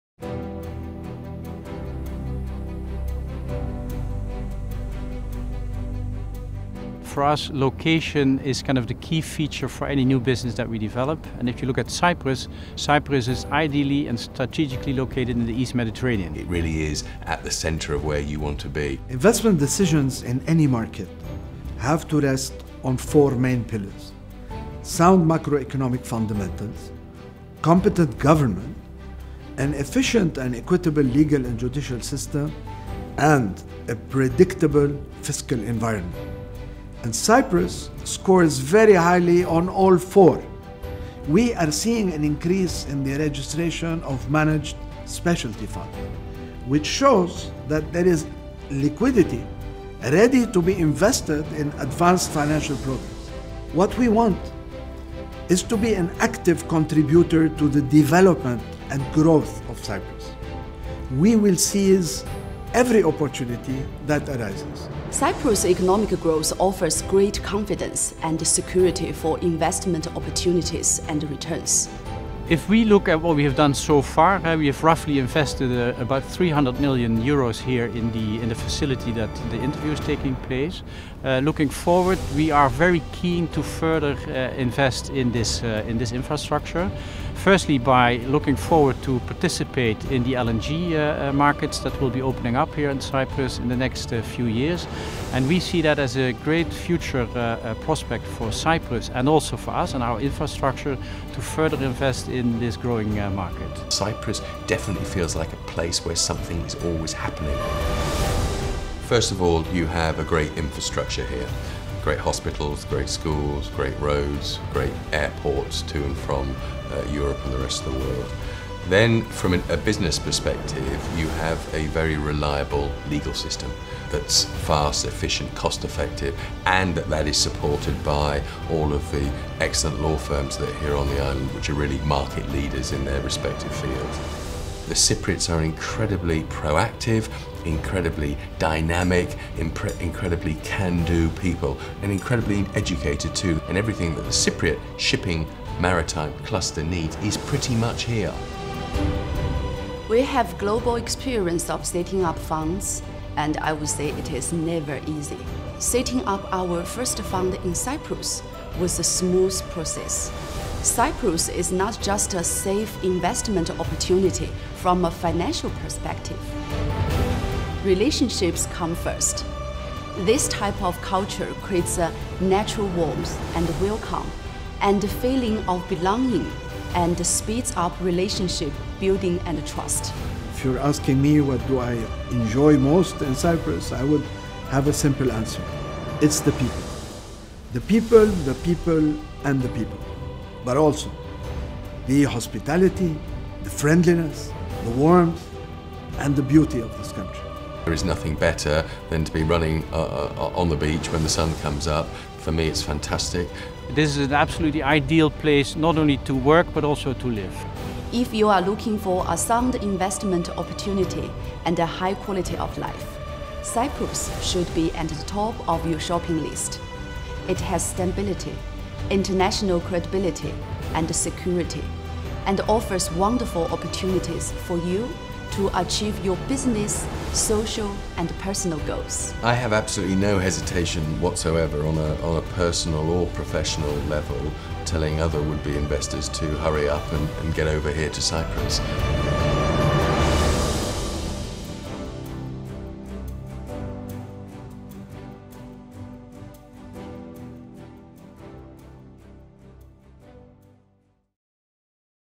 Invest Cyprus - CEO Interviews 2018
A number of notable CEOs of multinational companies based in Cyprus, share their business experiences in Cyprus.